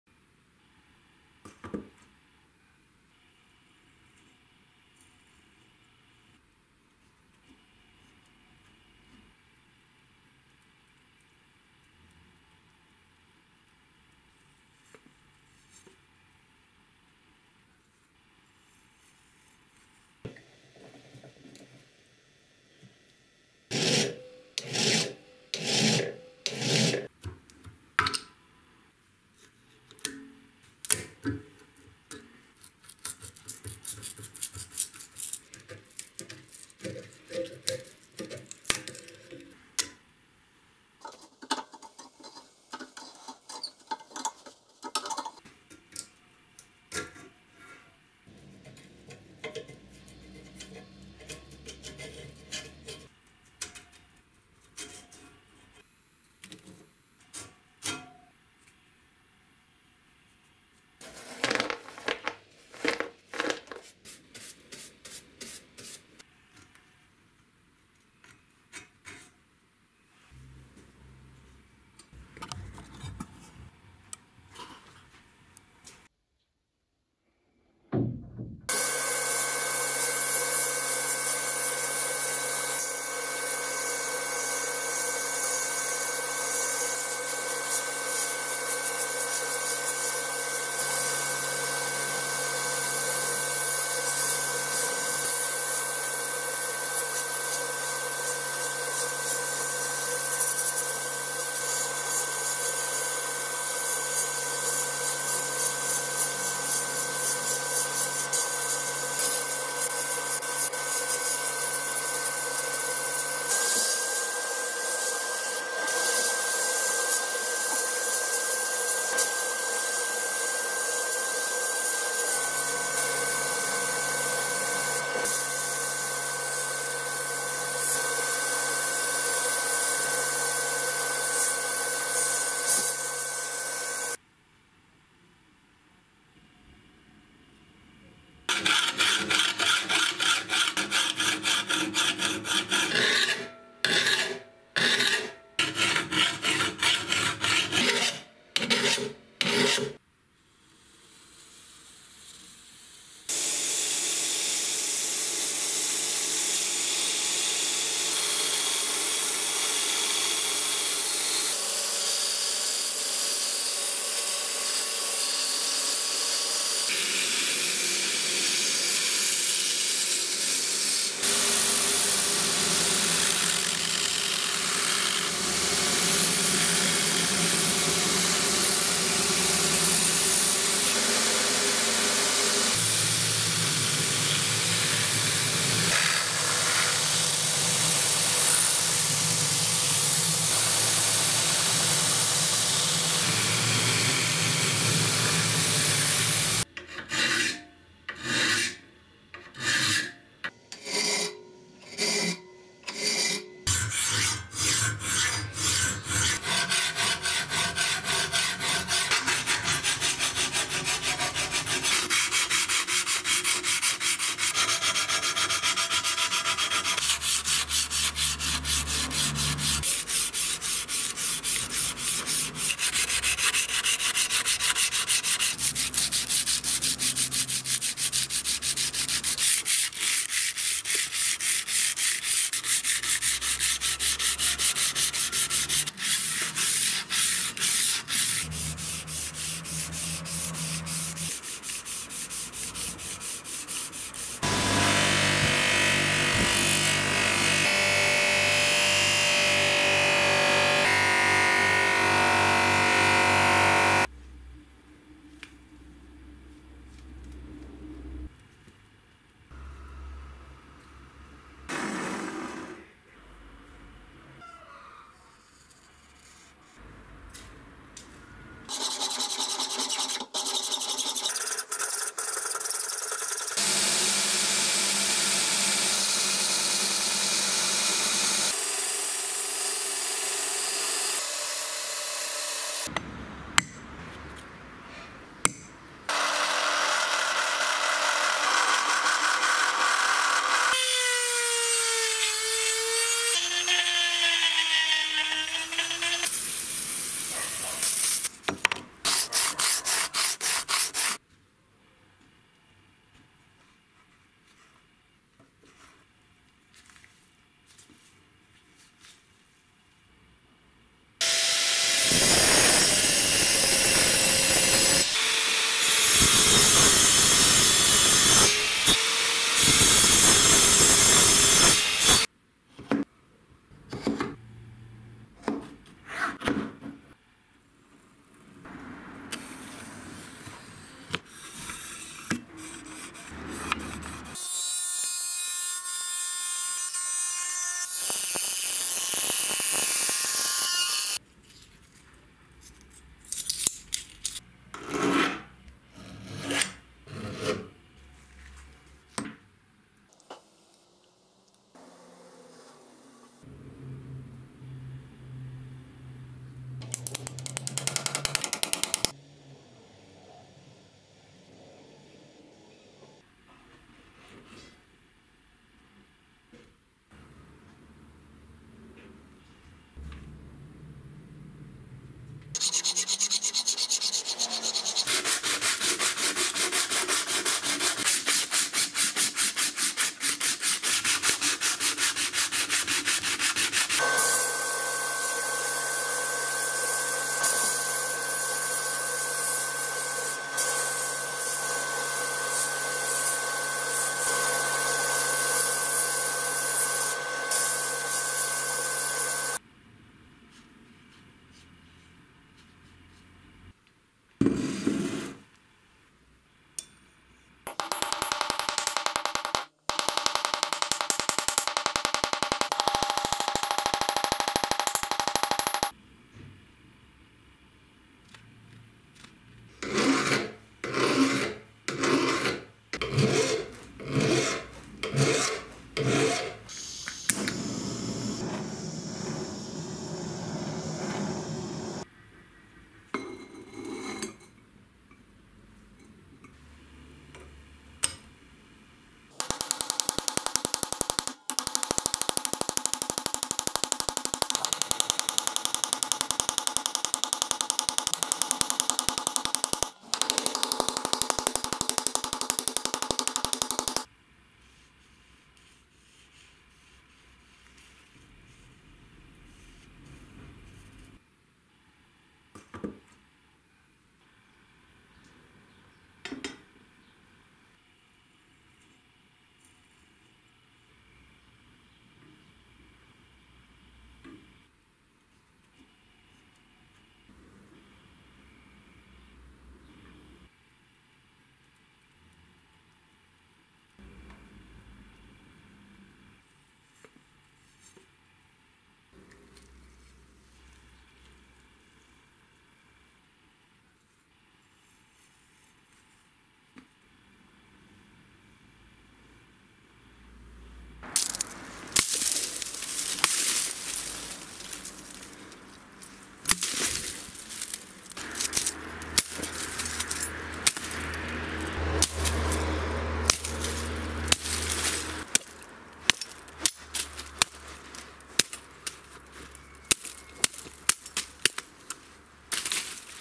Billhook Machete Restoration .
Hello
Today I bring you a restoration of a billhook bought at a flea market in Paris .There were traces of blows on the outside, the leather handle was very damaged. The rust had completely covered it.
I mounted the damaged pieces of leather and trashed it, then prepared the blade and metal part for sandblasting. After sandblasting, I used a file to remove the imperfections, then I sanded with a beld grinder . I polished the blade with sandpaper until it became a mirror .I started building the handle from scratch, at first I used a polished bronze piece until the mirror came, I continued to use pieces of aluminum and pieces of walnut wood joined together. Then I used tung oil for protection.
The last piece, after sandblasting, I straightened and applied hot bluing for protection .

My camera
Panasonic HC-VXF990